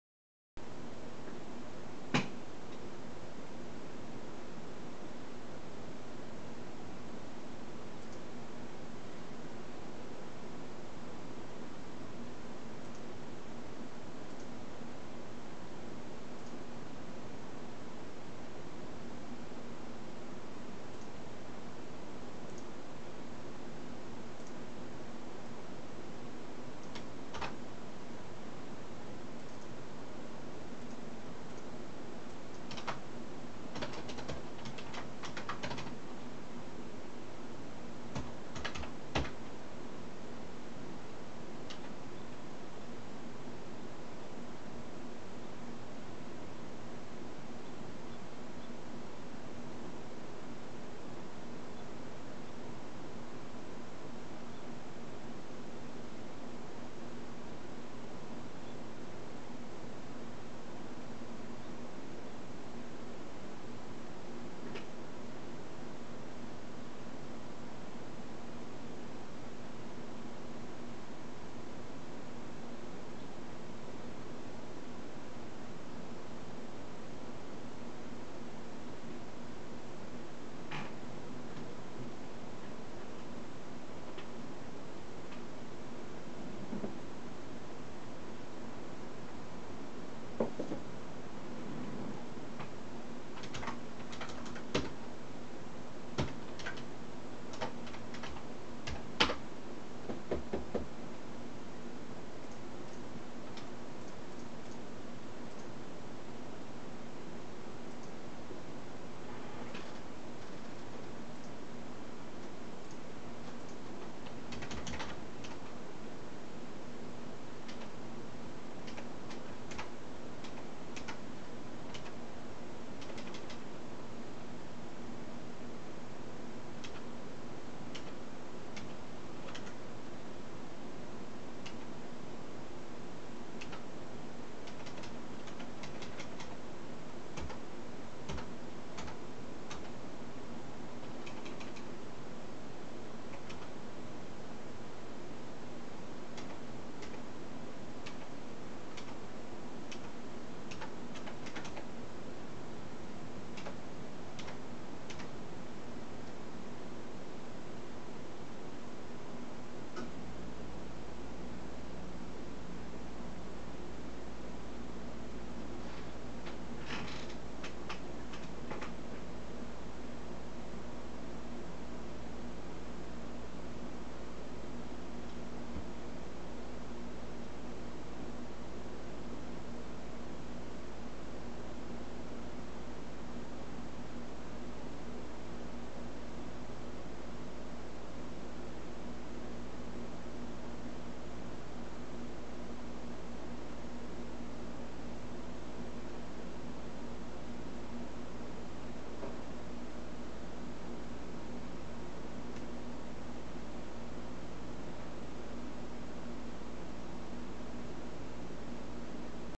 Cat dangling paw
Sadie enjoys being as lazy as possible, at all possible times.